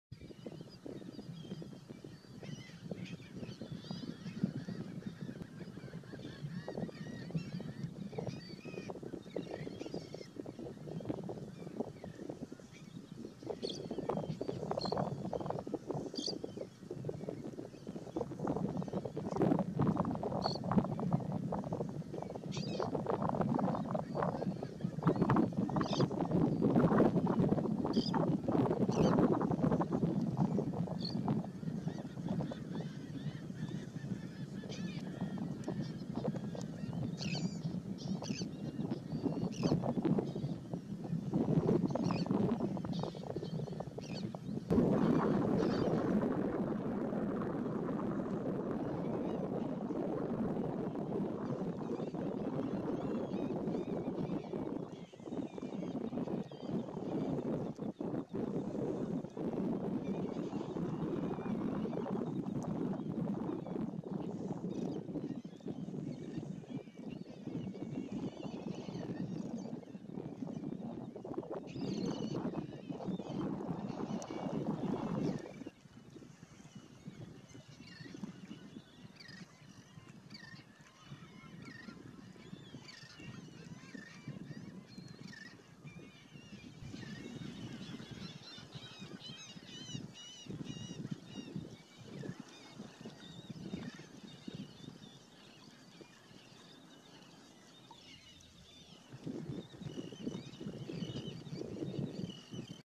American oystercatcher
Haematopus palliatus at Fort DeSoto, Florida. Late September, 2018.